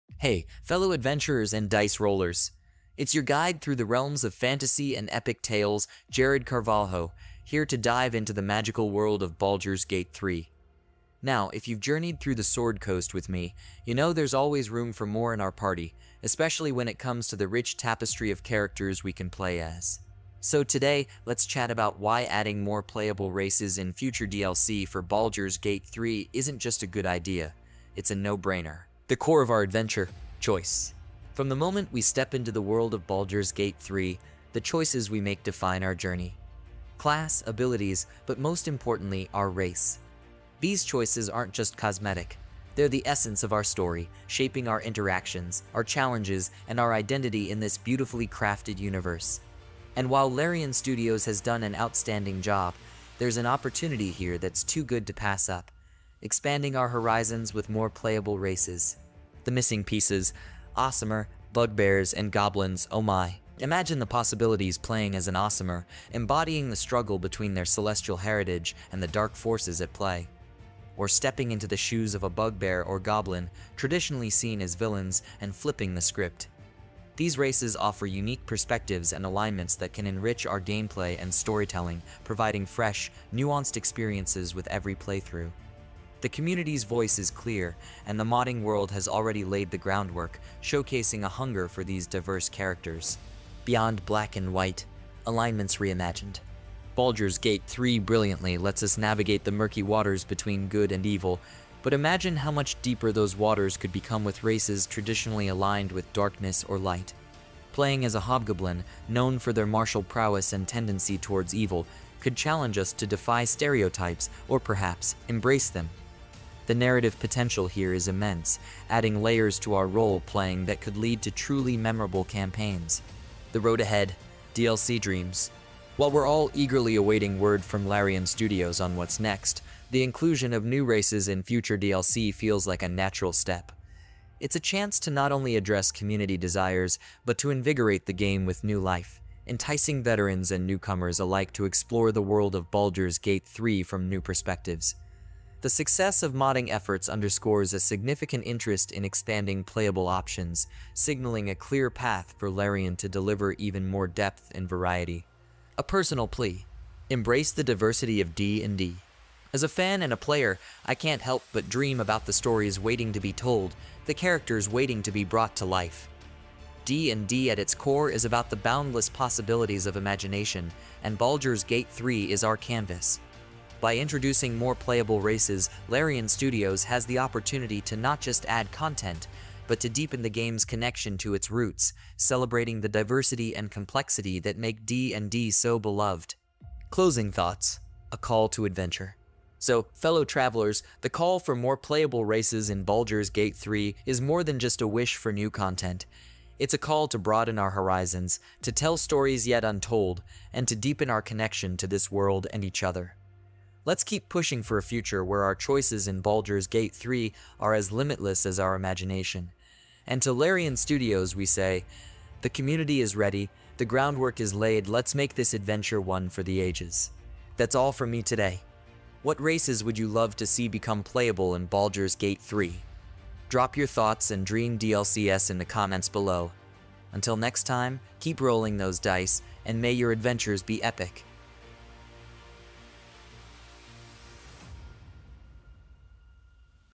Epic Reveal! New Playable Races Coming to Baldur's Gate 3? Dive Into the Future!
Dive deep with Jared Carvalho into the enchanting world of Baldur's Gate 3 as we explore the potential of new, thrilling playable races in upcoming DLCs. Are you ready to step into the shoes of an Aasimar, Bugbear, or even a Goblin, and flip the narrative on its head? Join us as we discuss why expanding the roster of races is not just a fantastic idea but a crucial evolution of our beloved adventure. BaldursGate3 GamingNews DLCUpdates FantasyGaming RPGGames DnDUniverse 🎲✨

🔮 What's Inside: 🔮

The Core of Our Adventure: How choice defines our journey.
The Missing Pieces: A closer look at Aasimar, Bugbears, and Goblins.
Beyond Black and White: The impact of new races on game narratives.
The Road Ahead: Speculating on future DLC dreams.
A Personal Plea: Embracing the diversity of the D&D universe.

Drop your dream DLCs in the comments: What races would you love to see?

👾 Join our adventure today, and let's make Baldur's Gate 3 an even richer experience together!